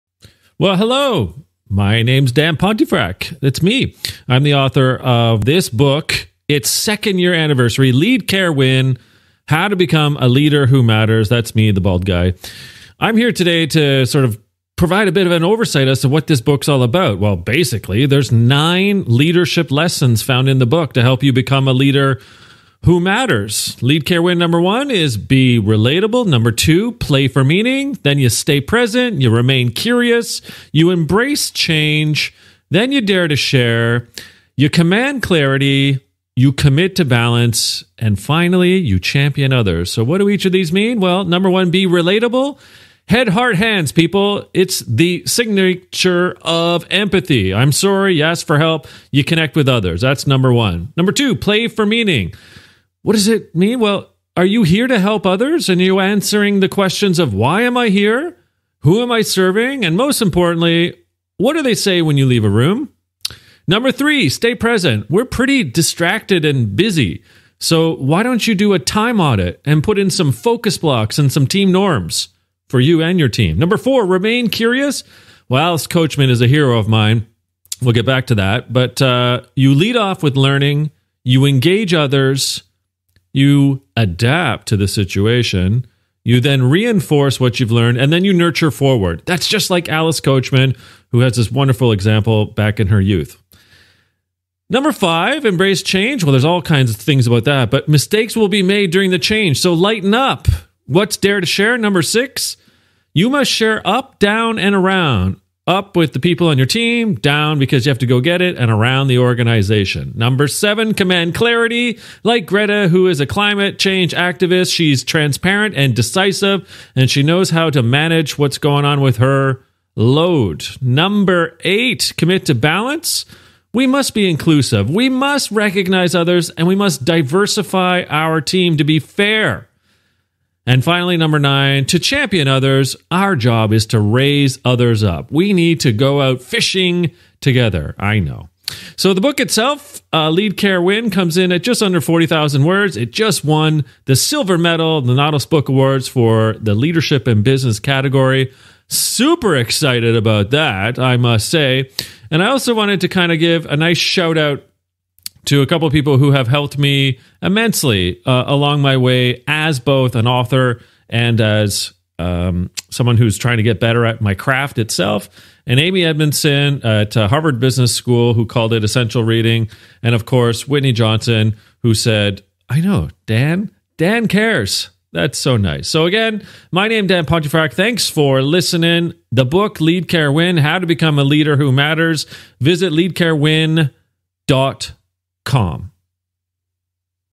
Lead. Care. Win. Animated
In this three-minute animated sketch, author Dan Pontefract takes you through the nine lessons that make up a leader who matters.

From his best-selling and award-winning book, "Lead. Care. Win. How to Become a Leader Who Matters," get a quick rundown of the nine leadership lessons that not only make up the book but what help to shape leaders into more humane, personable, and empathic people.